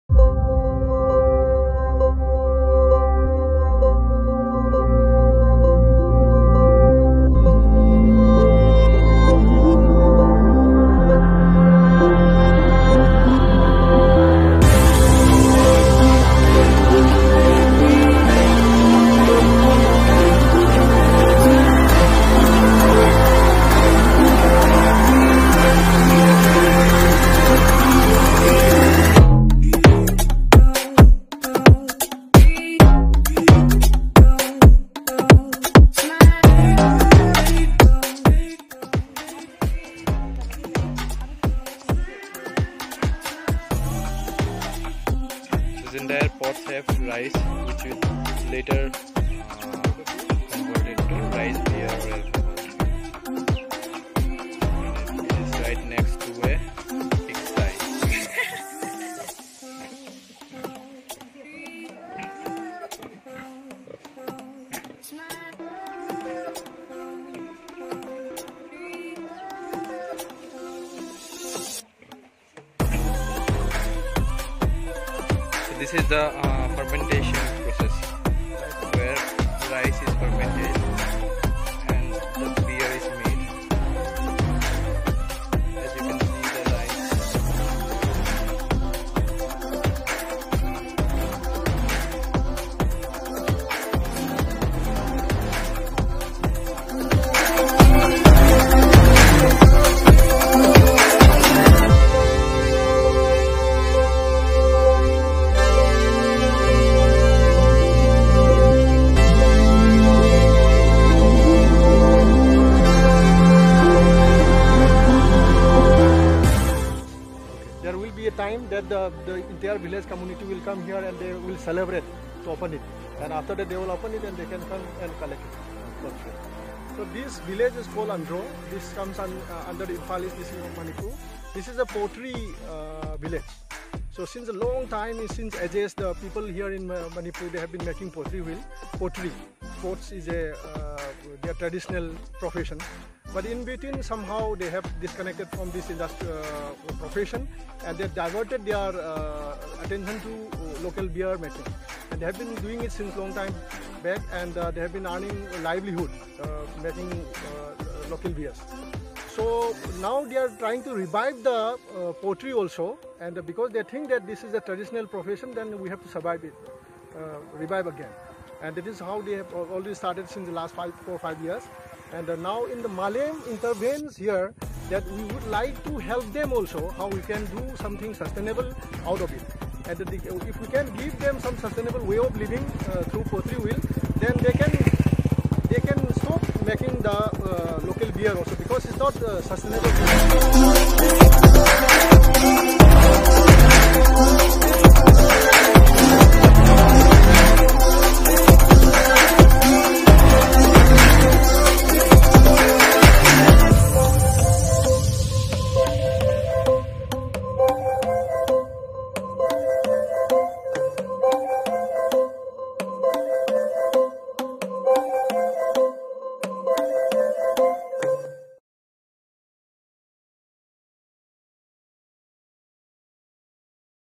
Potters from Andro Village
At a time when the king pushed all of Imphal valley to convert to Hinduism from the 1700s onwards, a small group of Meiteis chose to become social castaways in order to continue practicing their indigenous Sanamahi faith. An even smaller group of their descendants live in Andro village today, and still practice their prized heritage craft, the rare pottery form of Charai Taba.